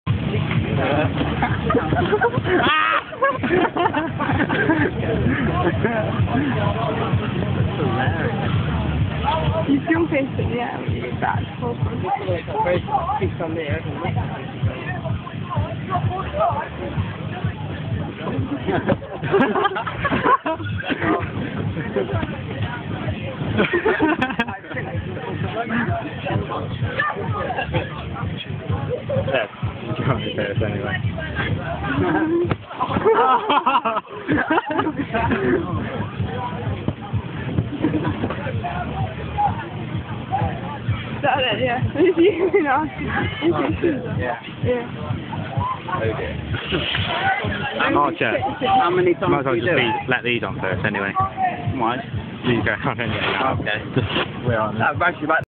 Tom & Matt Racing
They are on one of those inflatable games were you are attached to a bungee rope and it pulls you back!